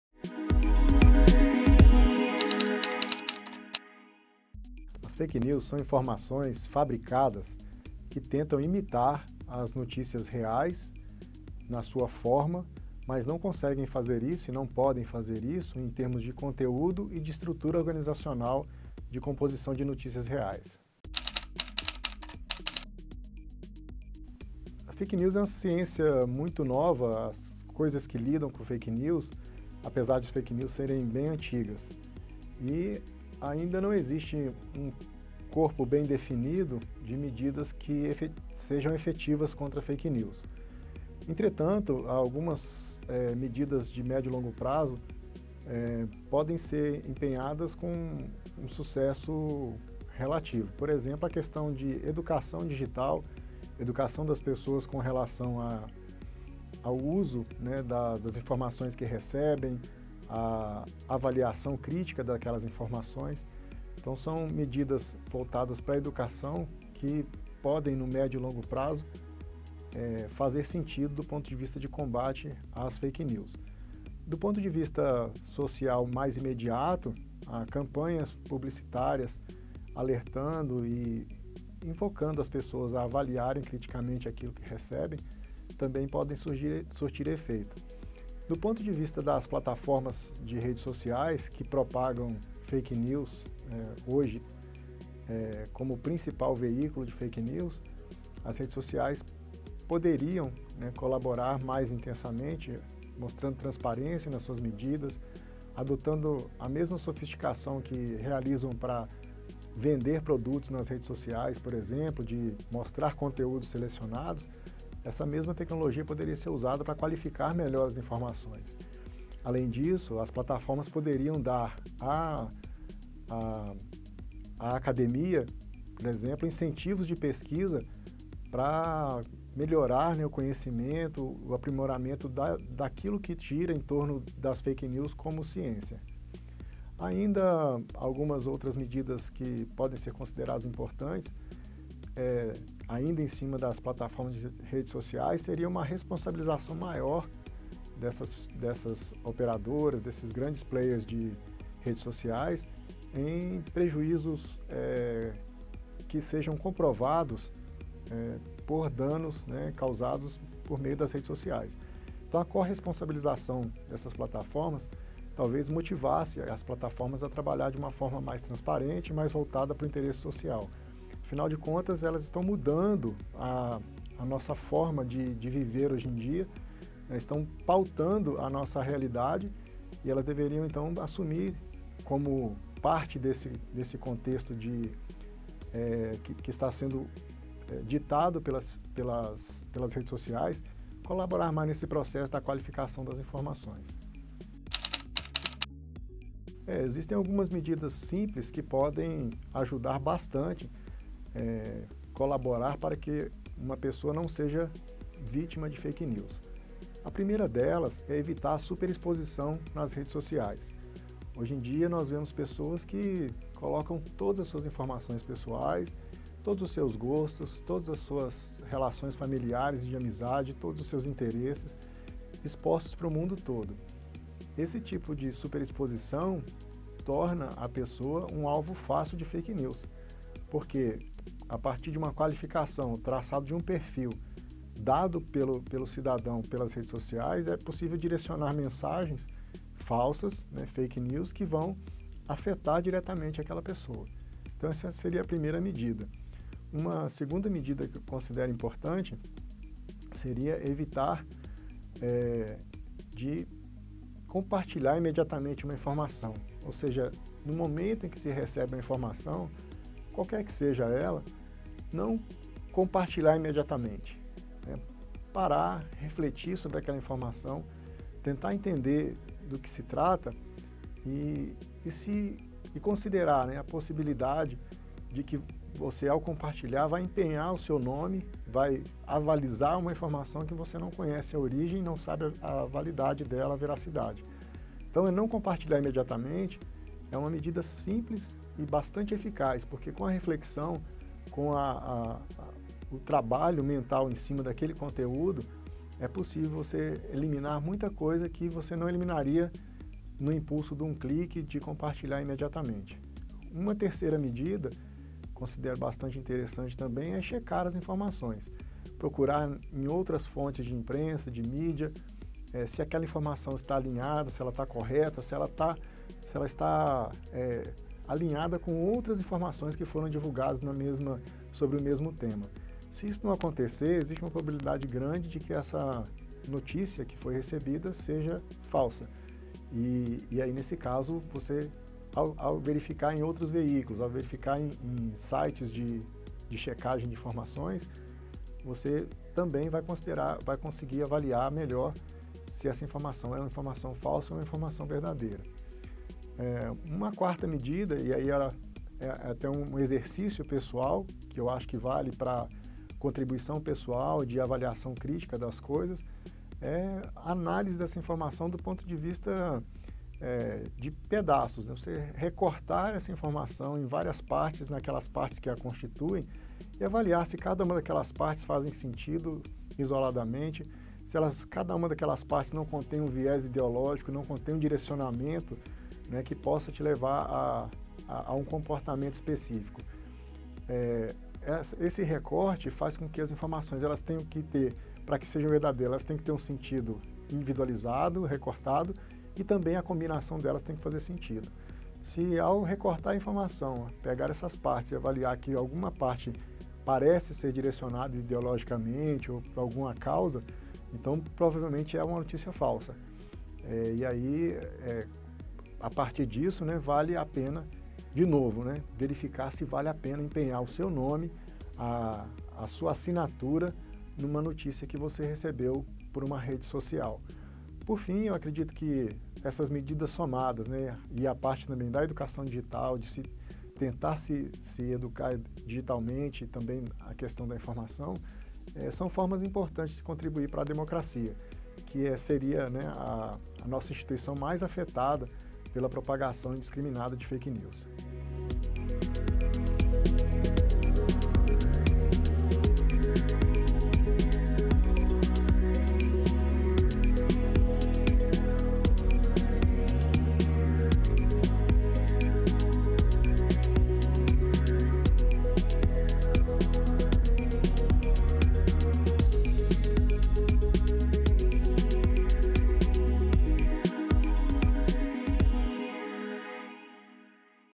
Com a palavra, o perito! "Fake News"
O perito criminal federal, Evandro Lorens, explica o que são as fake news e como combatê-las.